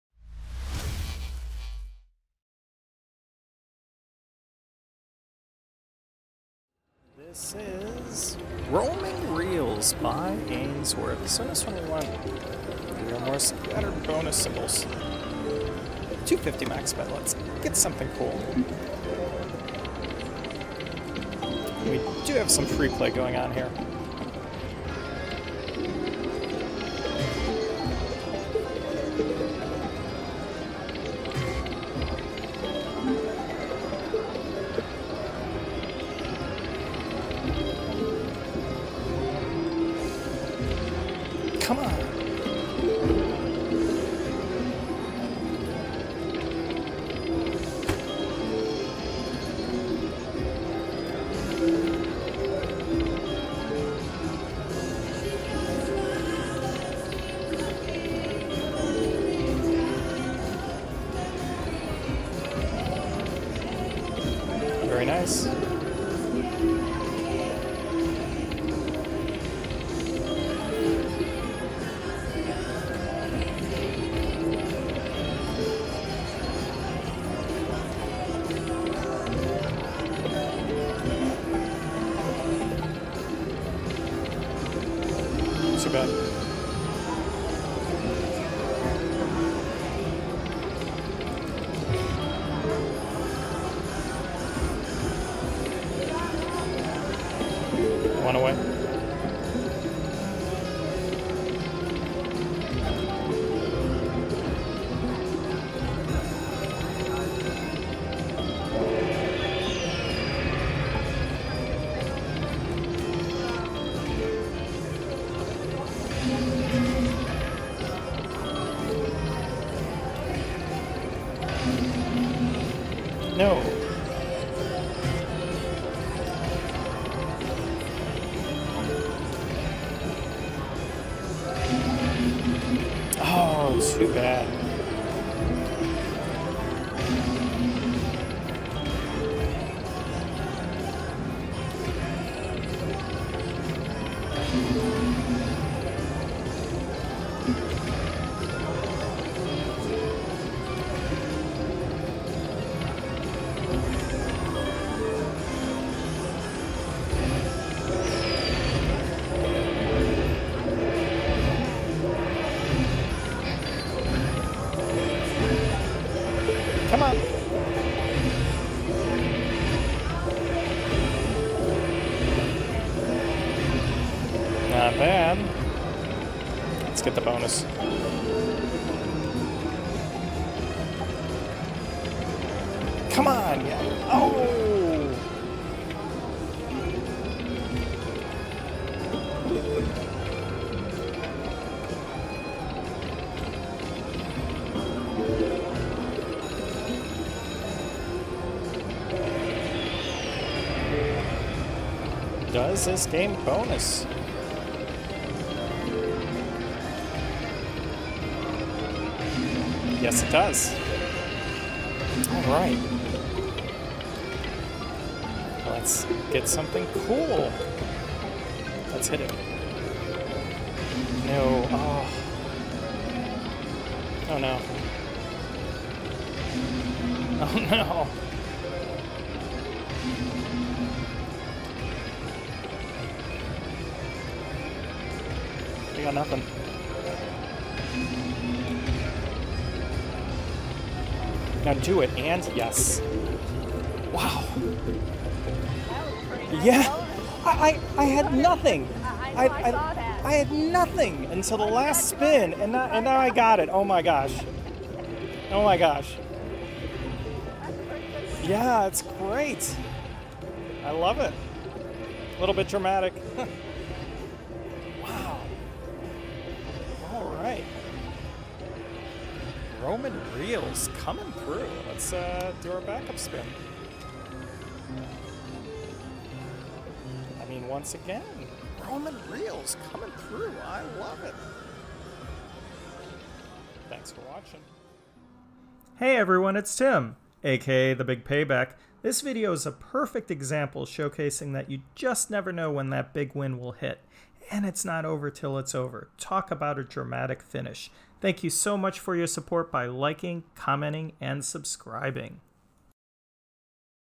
Roaming Reels Slot - BIG WIN BONUS!
THE TOP AWARD SCORED on the Roaming Reels slot machine by Ainsworth!

Roaming Reels is a fairly standard Ainsworth video slot where you win by landing winning combinations on paylines although the Big Win potential is in the bonus, which is triggered by landing 3, 4, or 5 bonus symbols for 8, 12 or 20 free games respectively.

During the bonus, a different set of reels is used and wild lion symbols are now stacked on reels 1, 3, and 5 only. The Paw symbol is also stacked on reels 2 thru 4 to give you a better shot at Filling the Screen. Retriggers are possible by landing 3 or more bonus symbols for 5 additional spins. All in all, what a great session on Freeplay, Enjoy!